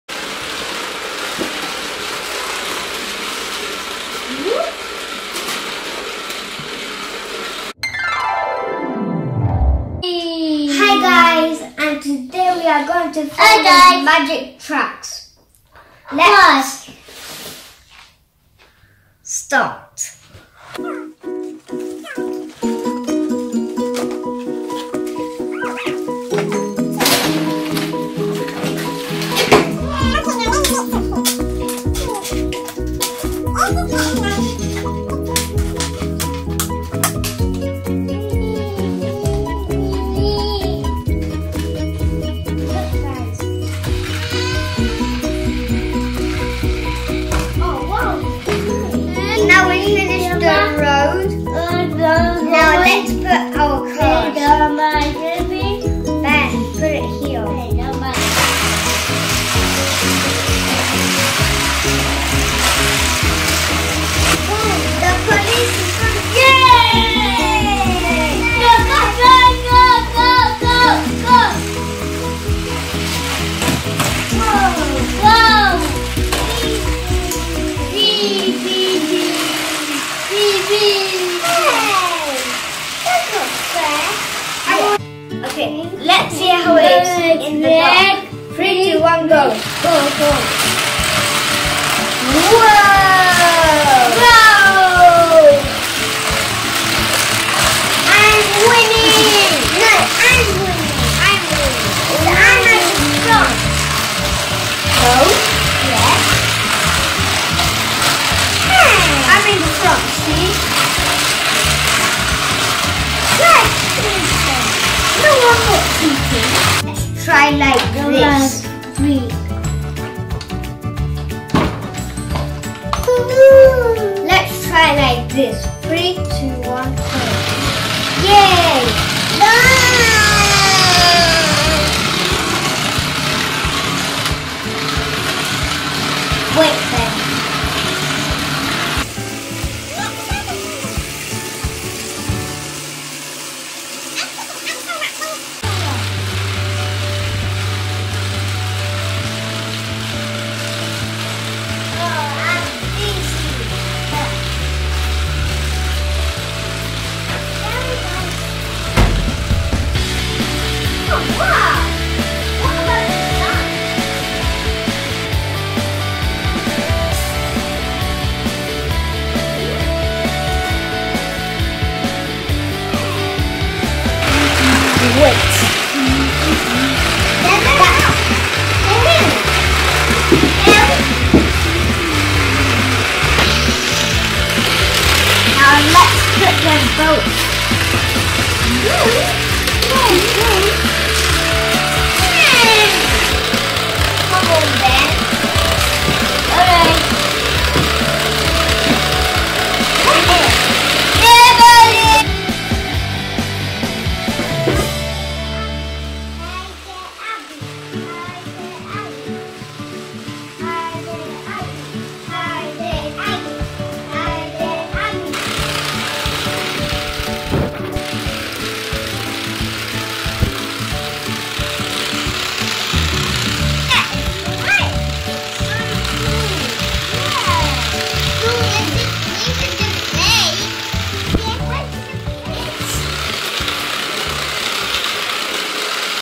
Crazy Magic Cars/Tracks Glow in The Dark/Kids race
Trying new toys,having fun with glow in the dark cars